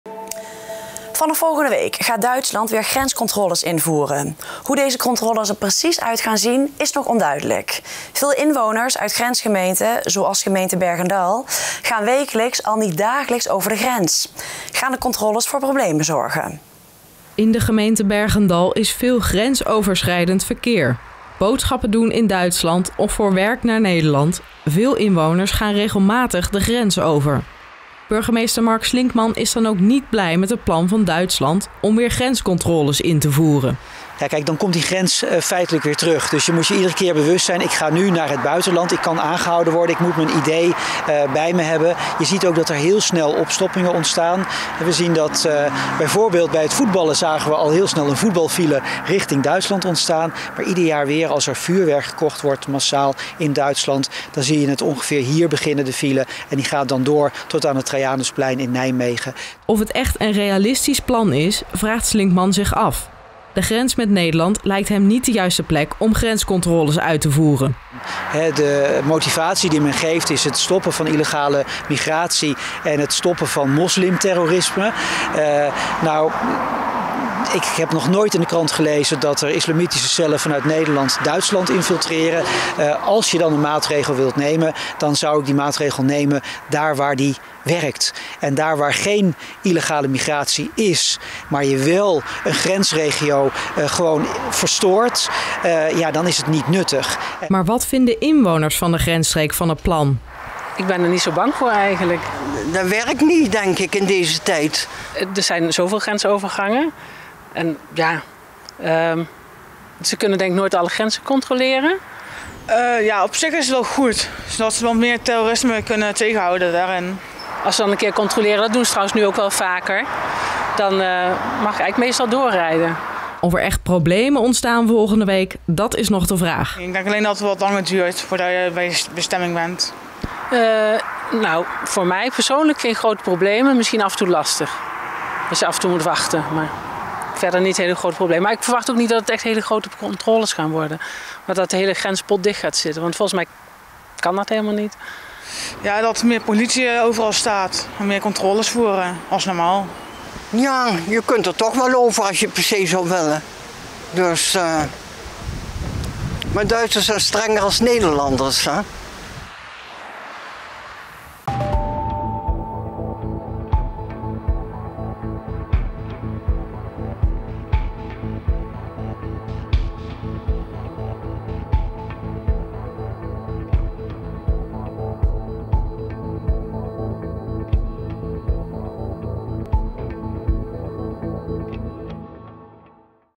Impact Duitse grenscontroles op verkeer omwonenden || RN7 REGIONIEUWS
Vanaf volgende week gaat Duitsland weer grenscontroles invoeren. Hoe deze controles er precies uit gaan zien is nog onduidelijk. Veel inwoners uit grensgemeenten, zoals gemeente Berg en Dal, gaan wekelijks al niet dagelijks over de grens. Gaan de controles voor problemen zorgen?